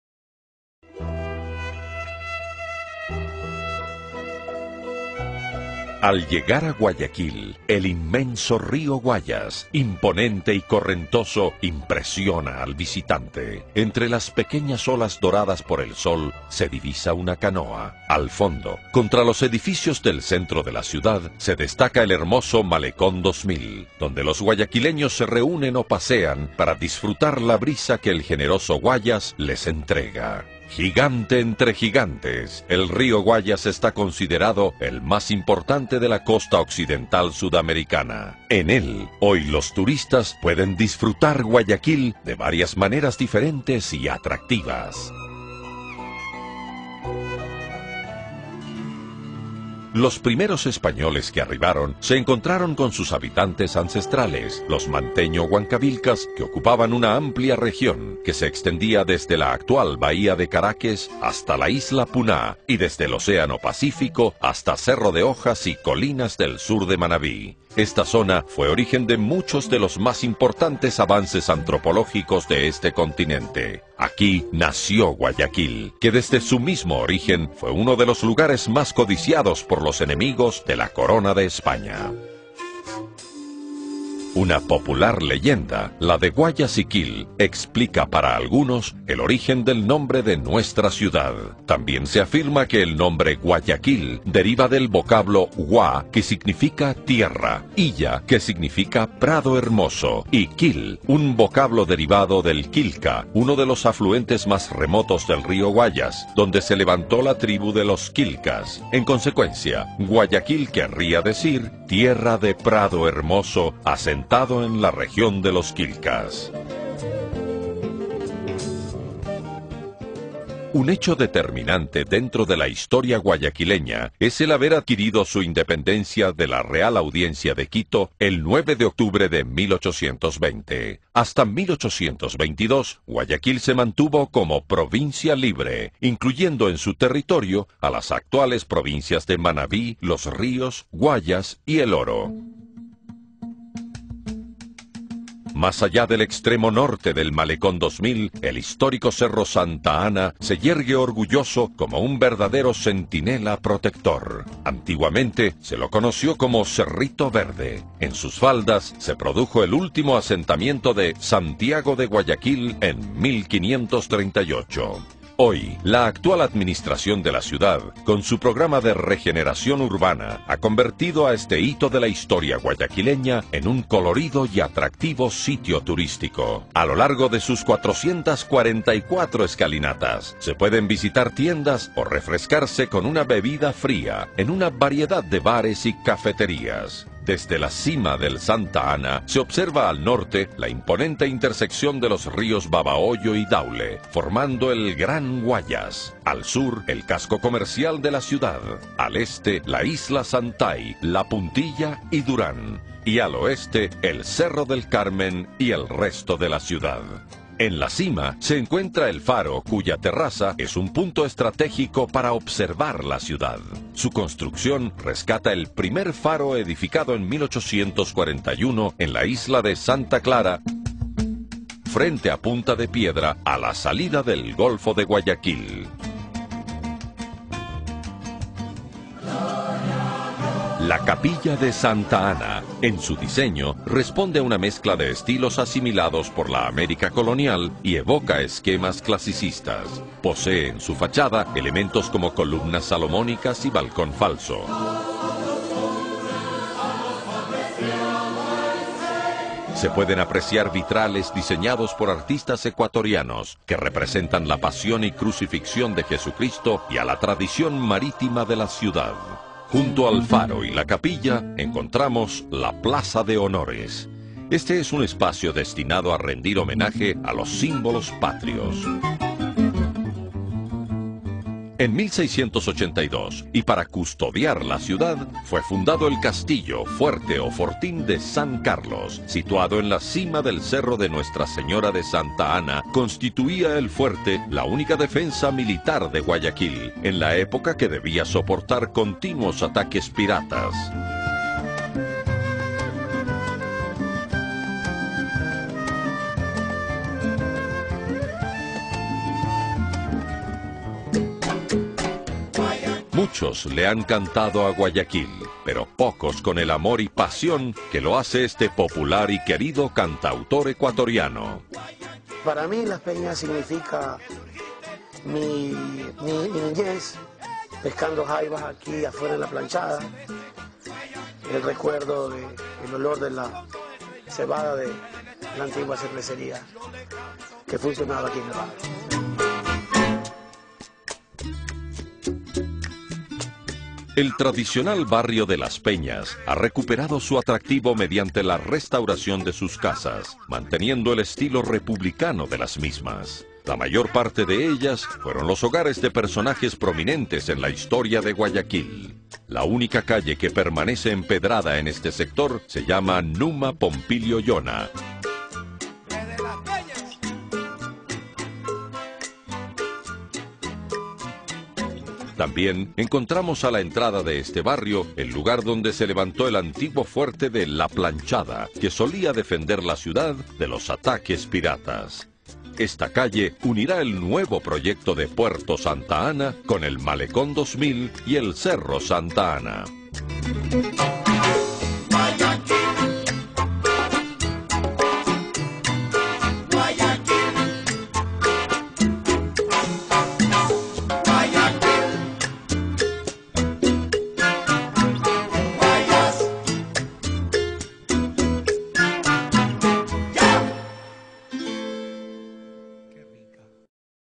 Guayaquil Documental Turistico (1-6)
Video promocional de Guayaquil La Perla del Pacifico.